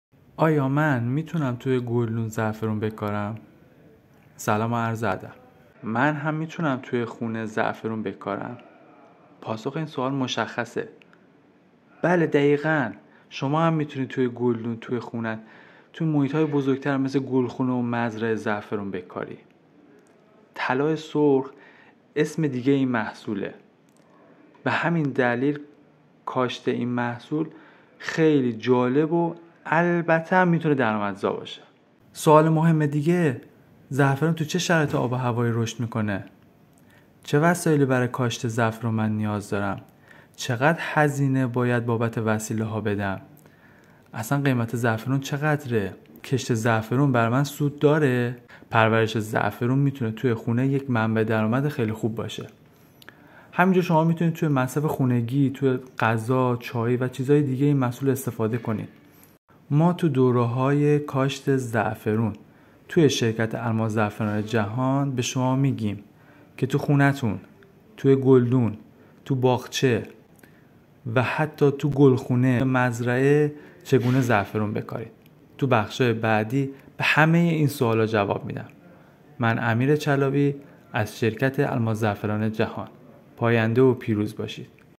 Growing saffron in a pot
Growing saffron indoors for your own consumption
Training courses on planting saffron in pots
Teaching how to plant saffron in a pot
Saffron King Business Company
SaffronKiing.com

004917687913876